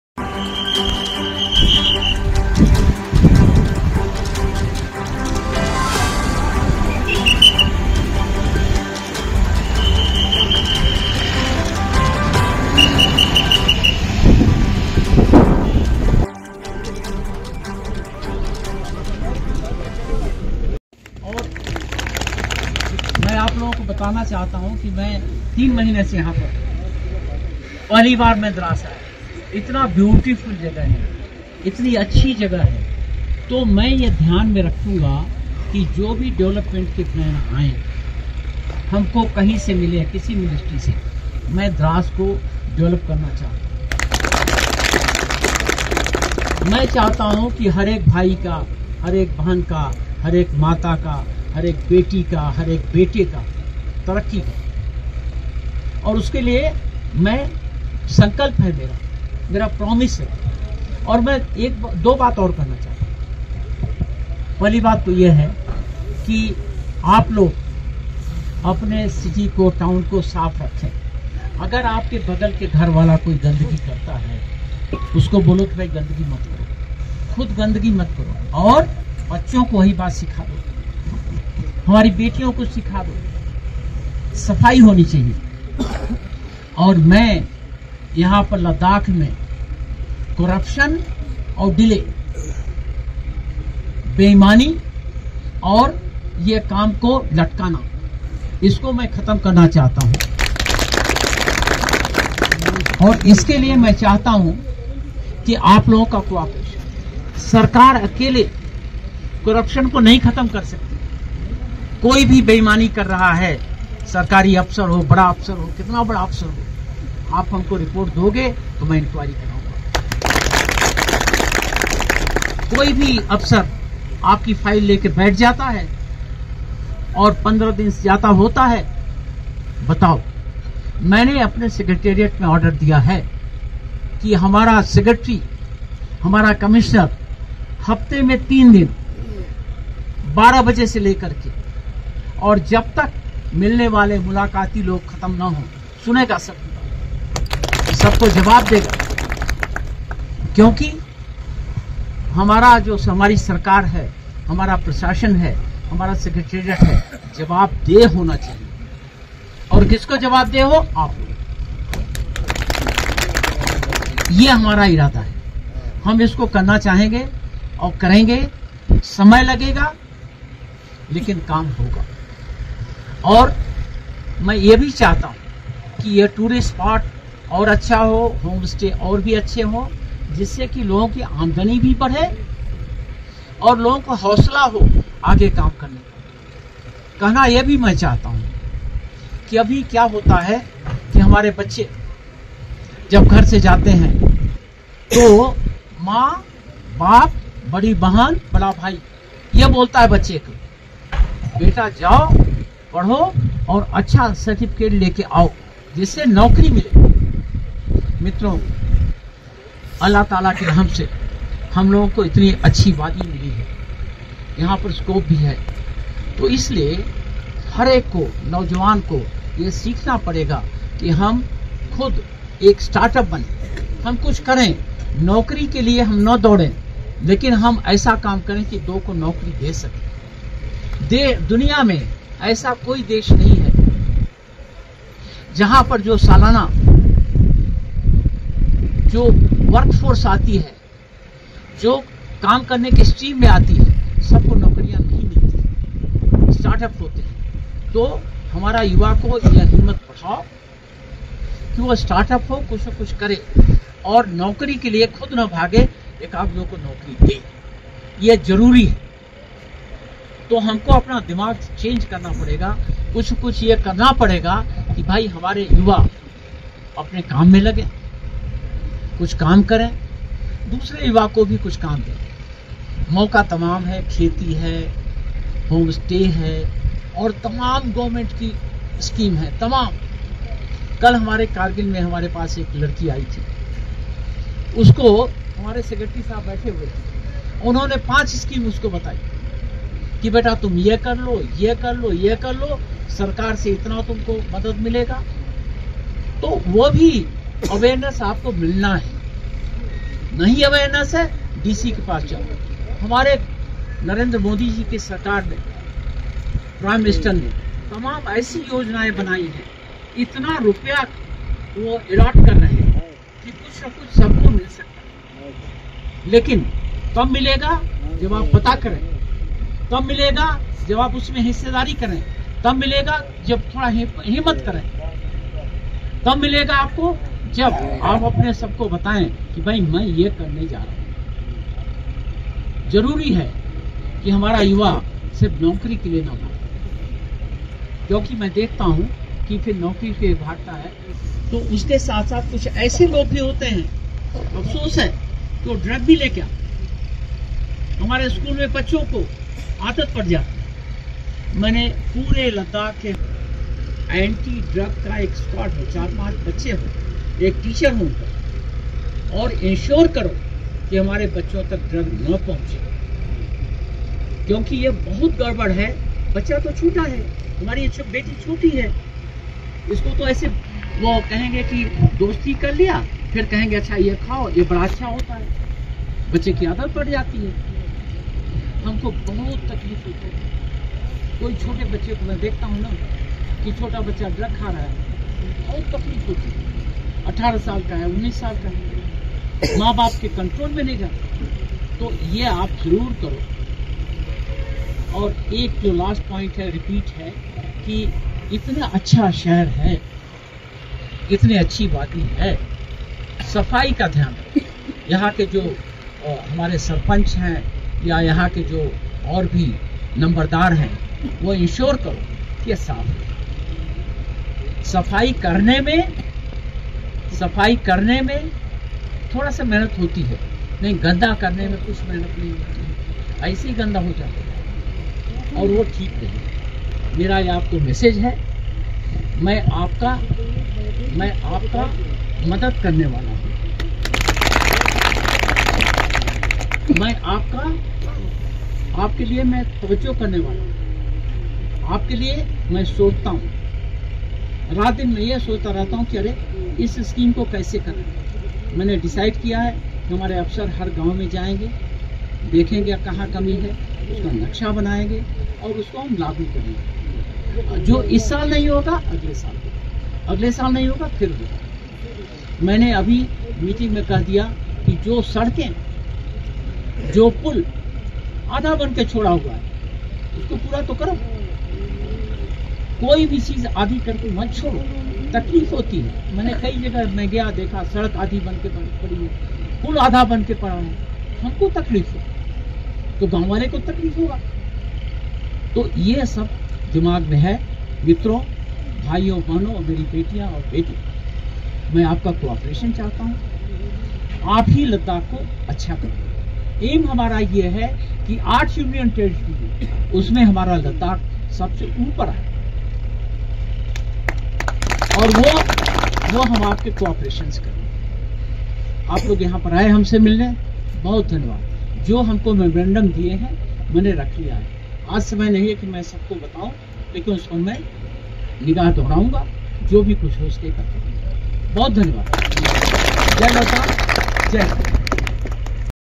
LG Ladakh, Honourable B.D Mishra visits Drass Sub Division
LG Ladakh, Honourable B.D Mishra visits Drass and met with people at Dak Banglow Drass.